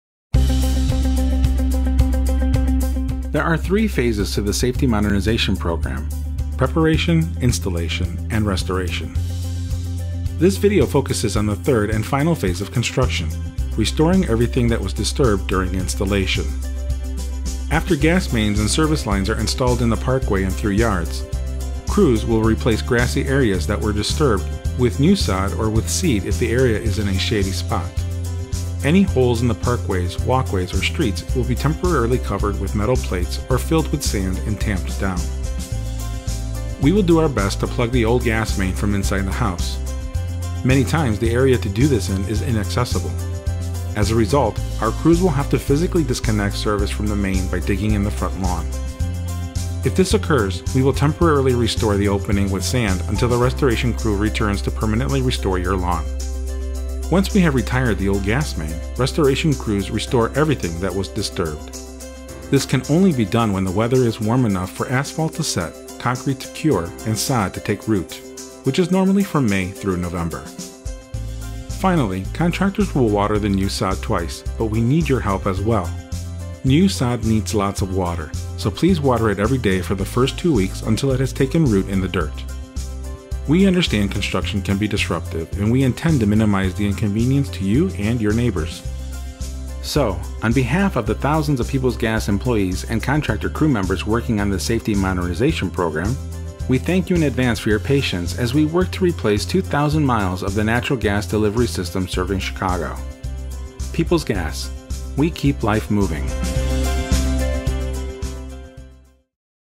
Safety Modernization Program: Phase 3 - Restoration (English)
The Peoples Gas Safety Modernization Program (SMP) is a safety initiative to replace more than 2,000 miles of deteriorating natural gas pipes, some of which date to the time of Abraham Lincoln. Learn about the restoration process.